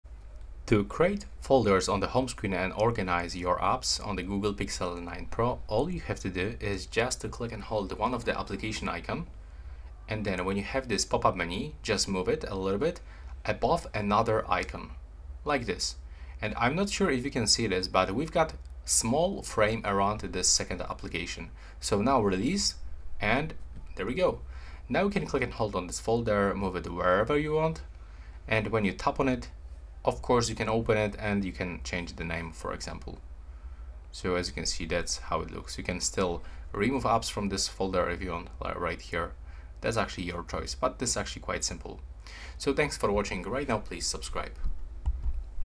How to Create Folders on GOOGLE Pixel 9 Pro - Home Screen Folders
Learn how to create folders on the Google Pixel 9 Pro. This video shows you how to organize your apps by grouping them into folders on your Pixel 9 Pro’s home screen. Easily set up and manage home screen folders on your Pixel 9 Pro for a more organized and efficient experience!

I'll do my best to respond promptly and assist you.